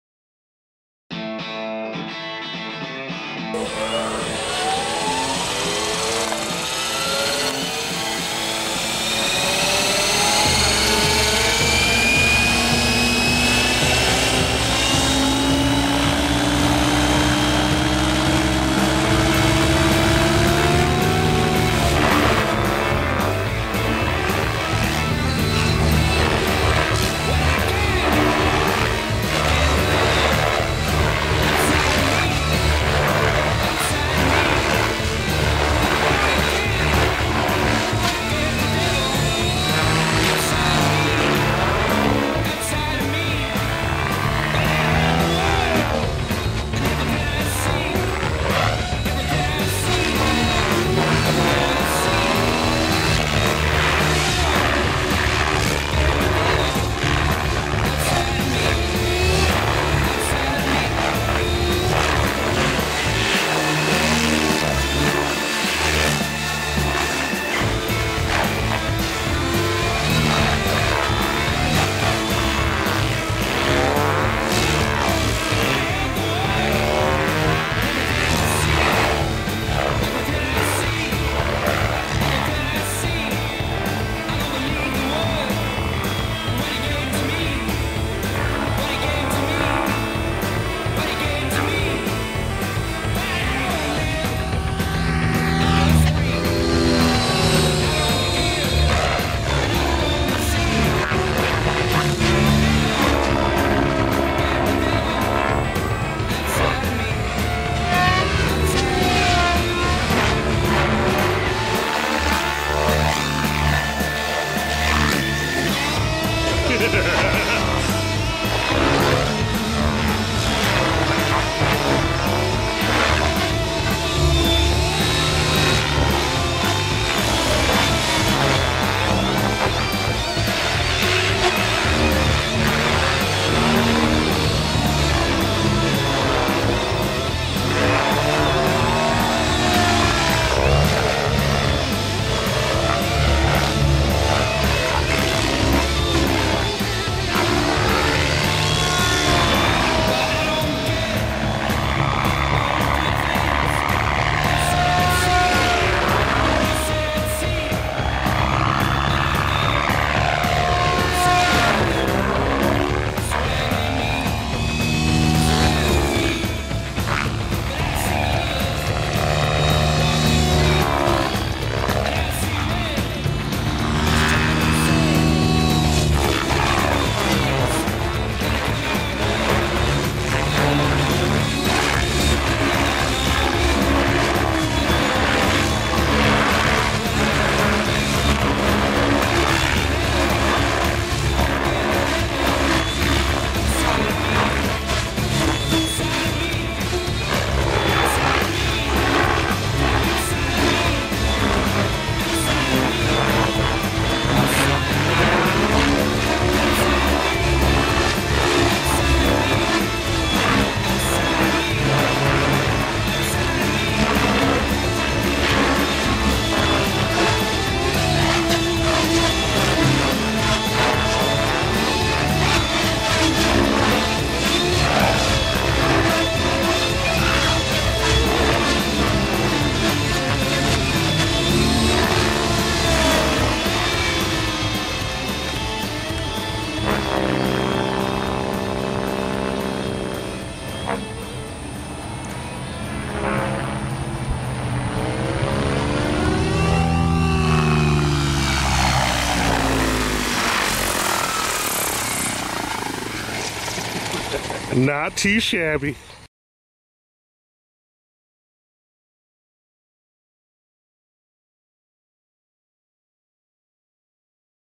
JC Zankl rocking the Gaui X7
Team Gaui's JC Zankl beating the air into submisision.